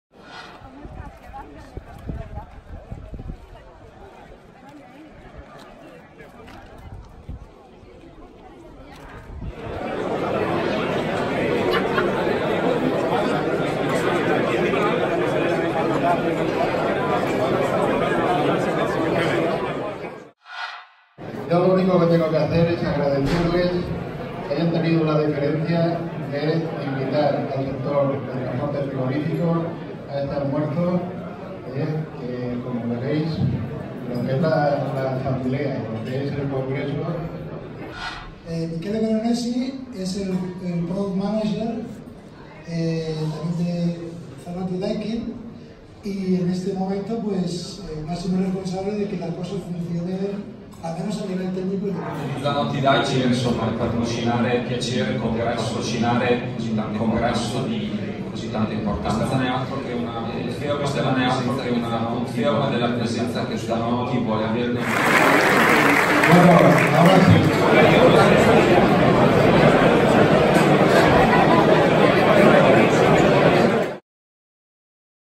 COMIDA INAUGURAL DEL V CONGRESO NACIONAL DEL TRANSPORTE FRIGORÍFICO
Zanotti Appliance ha patrocinado la comida inaugural del V Congreso Nacional del Transporte Frigorífico. Importante evento que reune a unas 300 personas del sector.
Organizado por @ATFRIE es una de las citas más destacadas. Las personalidades más destacadas debaten los principales temas del transporte frigorífico por carretera.
Por primera vez, Zanotti participa en este Congreso Nacional como patrocinador. Gran satisfacción con el resultado obtenido. Fraancamente muy satsifechos con la gestión de ATFRIE.